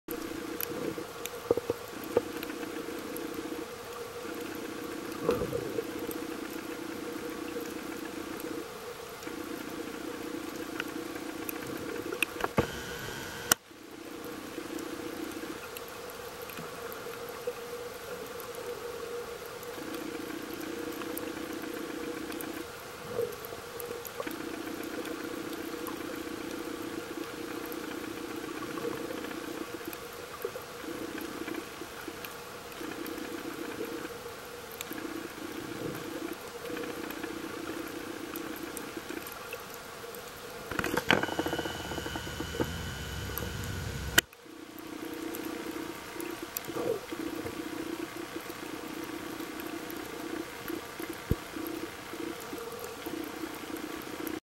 Поедание медузы
Рыбы с удовольствием едят медузу